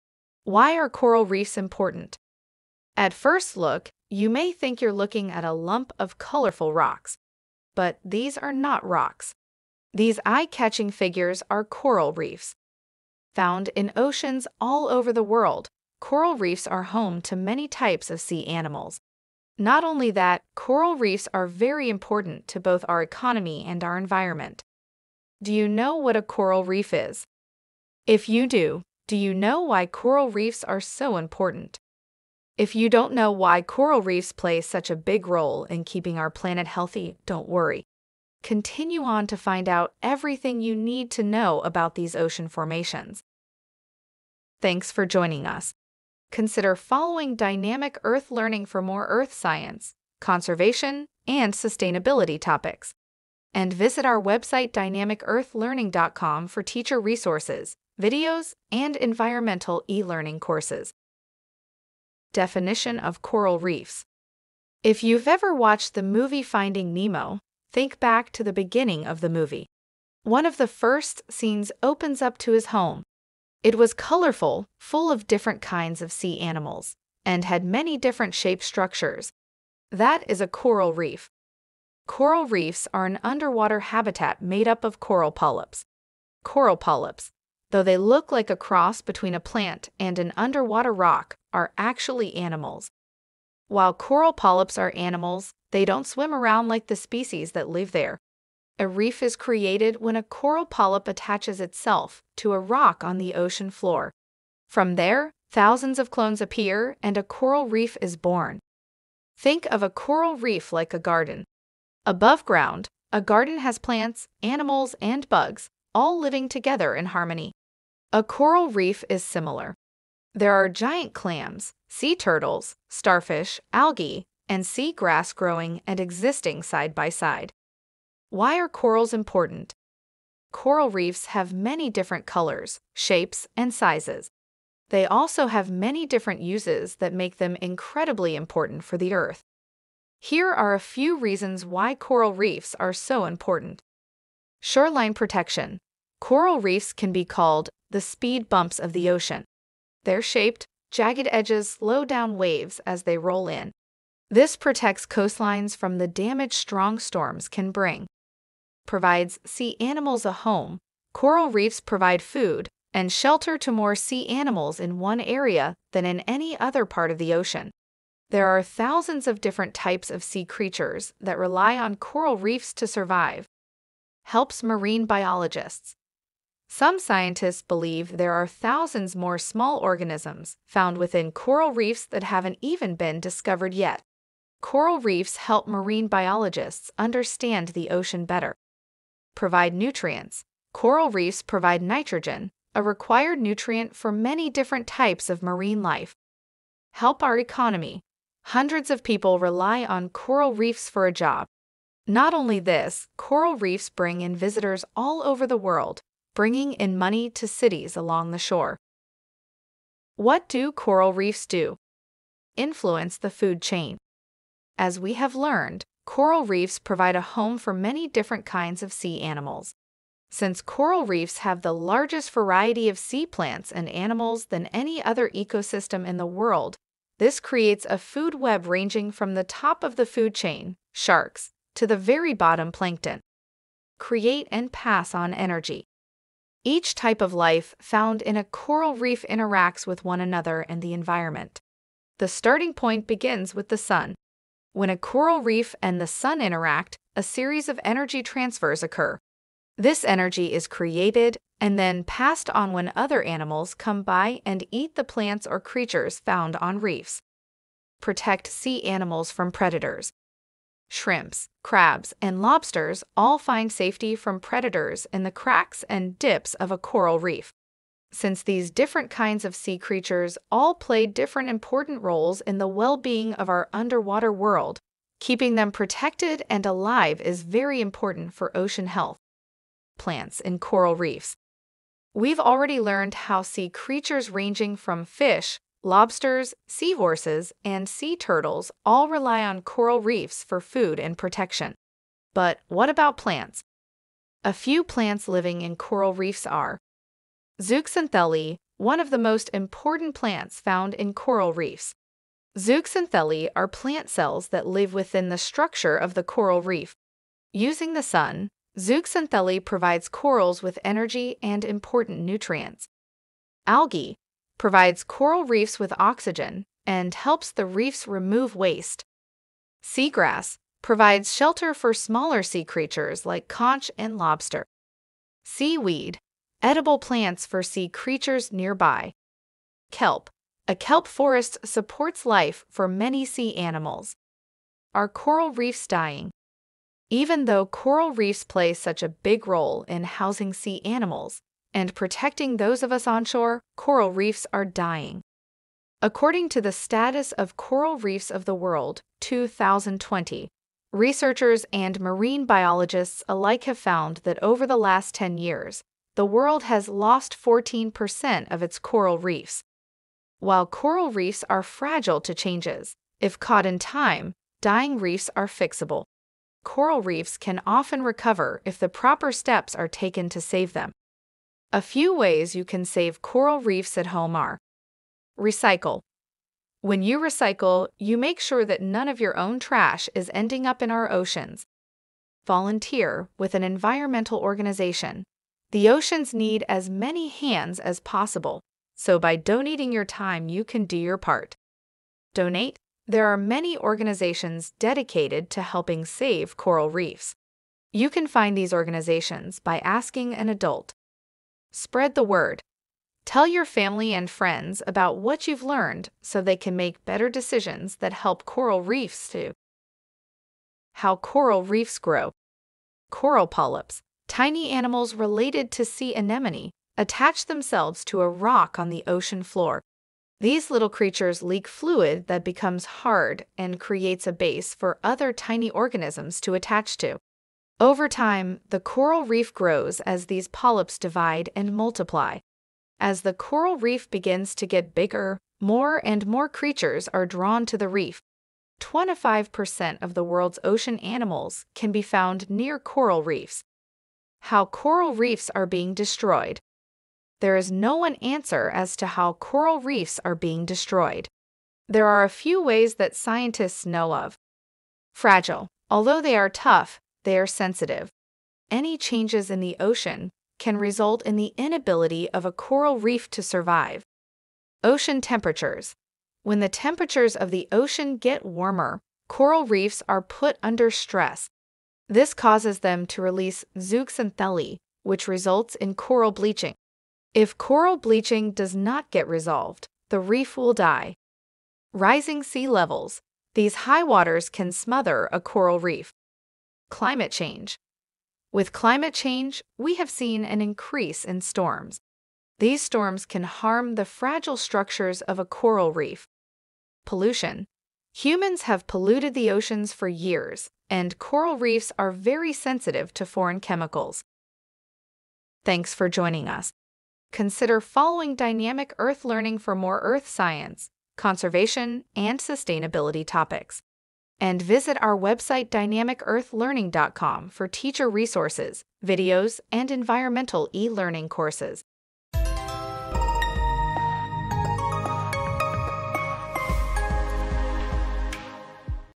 Why Are Coral Reefs Important? Ocean Sustainability
Why Are Coral Reefs Important? This ocean sustainability video covers
definition of coral reefs, why are corals important, what coral reefs do,
plants in coral reefs, are coral reefs dying, how coral reefs grow, and how coral reefs are being destroyed.

🖳 See Dynamic Earth Learning's online environmental science courses!